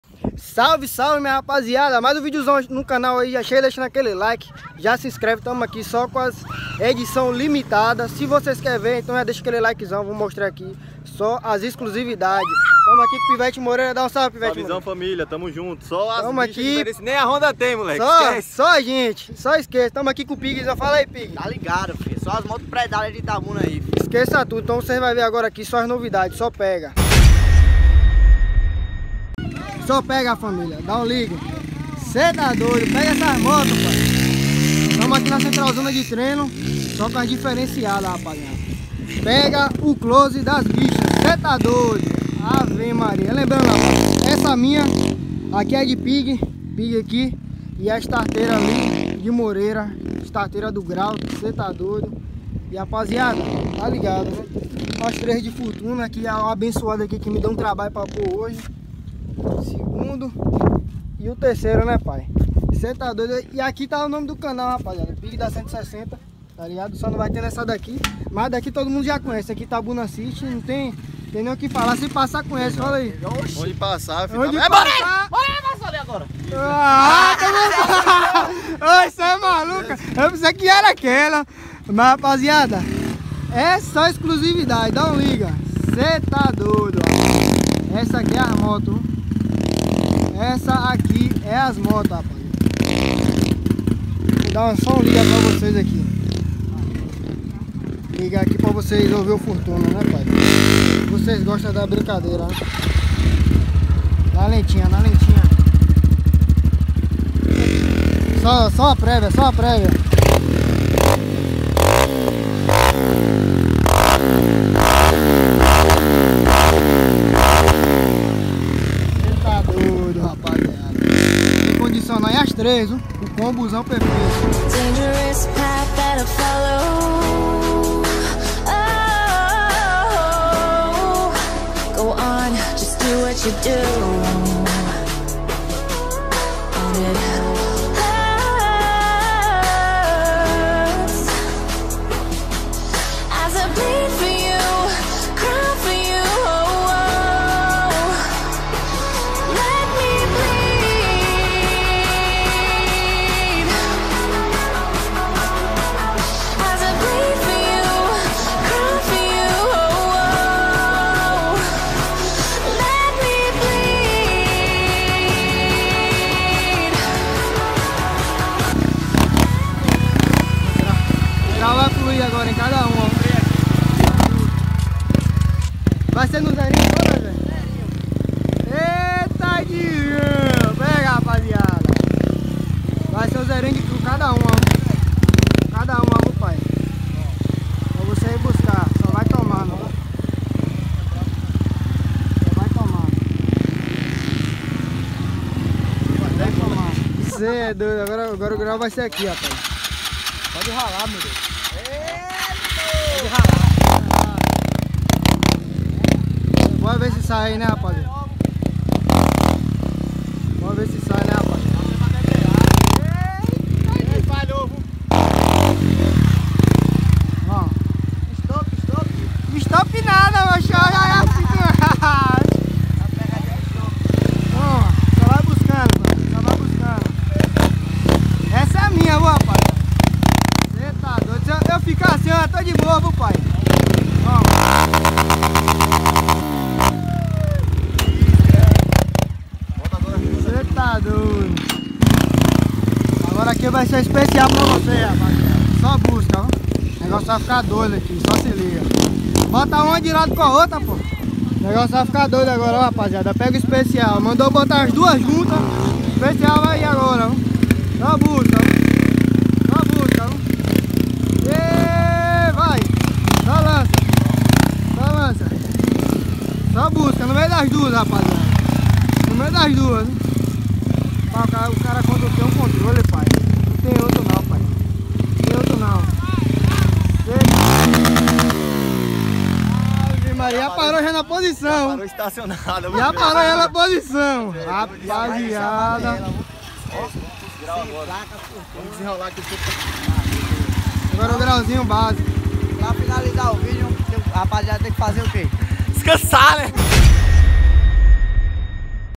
AS 160 MAIS CHAVE DO YOUTUBE BR 😍 - VEJA OS DETALHES
Modo drao ativado, aquele motovlog brabo pra vocês acelerando forte com escape torbal estralador, naquela pegada.

00:00 ESTRALA NO LIKE😝
1:30 INSCREVA-SE, SEJA BEM-VINDO❤️